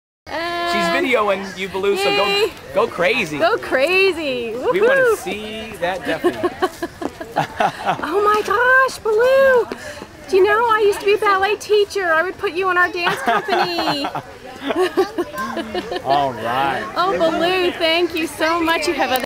EPCOT Baloo dancing with Duffy the Disney Bear
Duffy the Disney Bear meets the Jungle Book's Baloo in EPCOT.